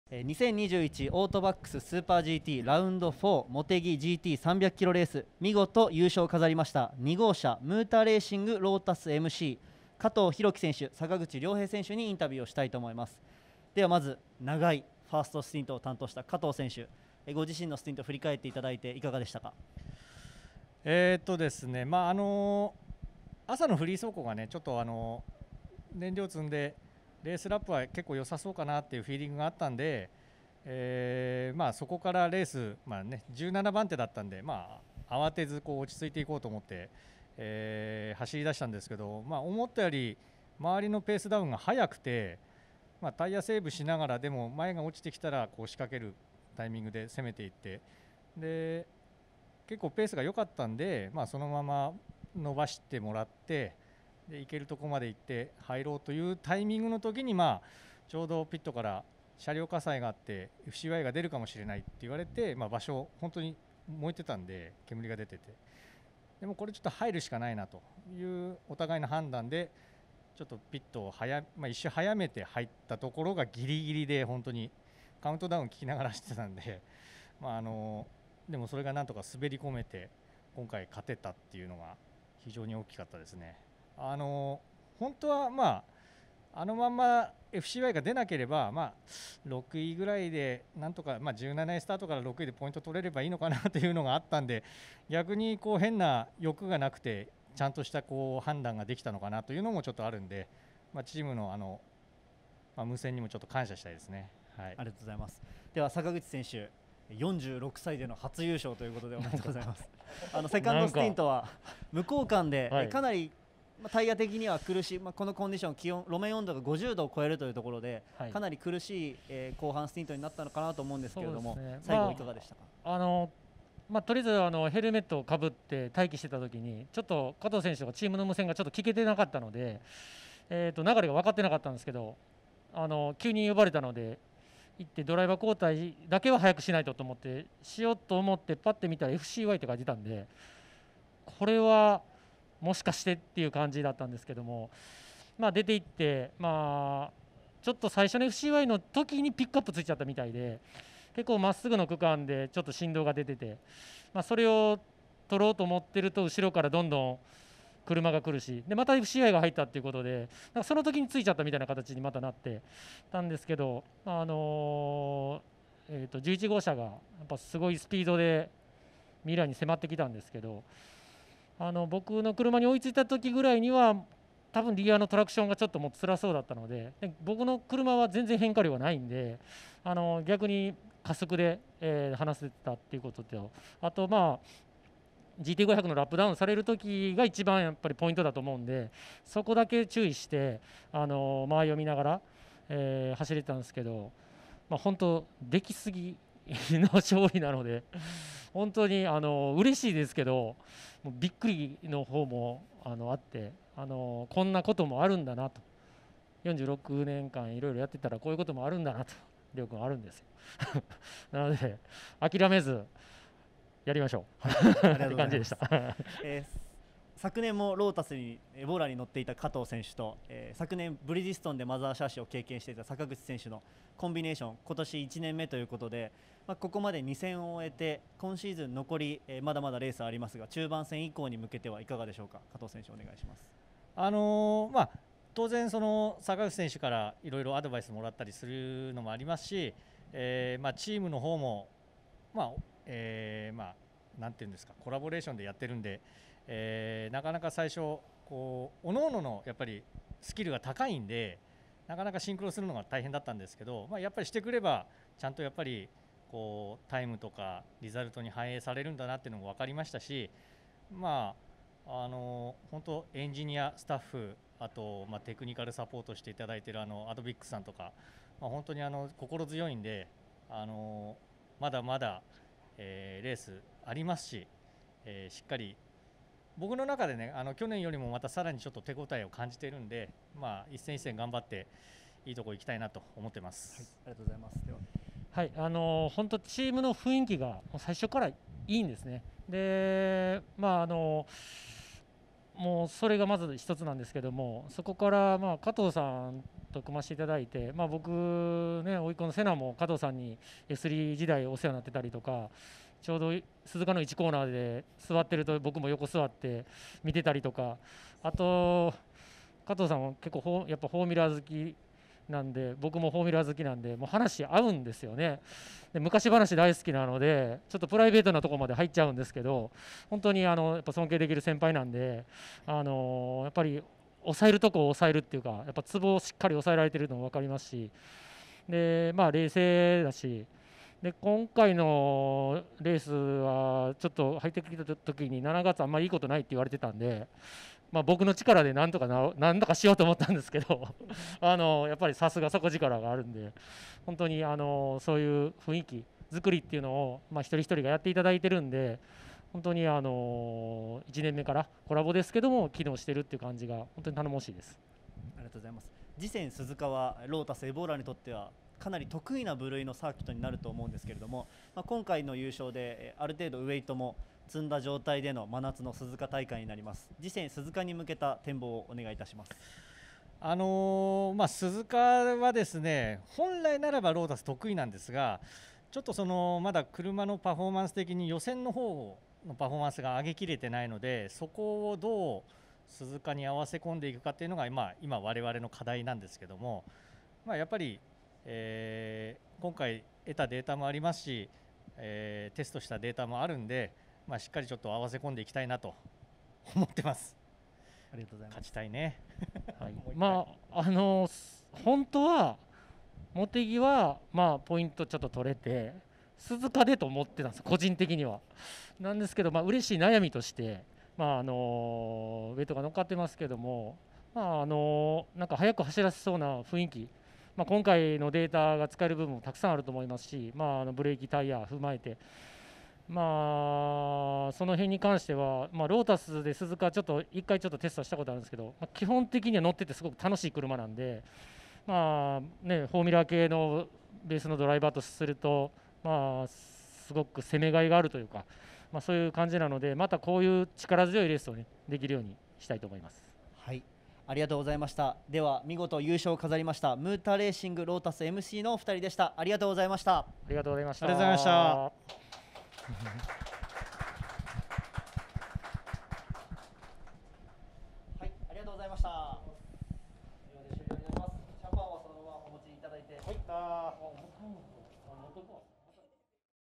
【優勝ドライバーインタビュー/GT300】加藤寛規／阪口良平 No.2 muta Racing Lotus MC 2021 Rd.4 もてぎ
2021 AUTOBACS SUPER GT Round4
もてぎ GT 300km RACE

J SPORTSではSUPER GT 2021全戦の予選・決勝を放送！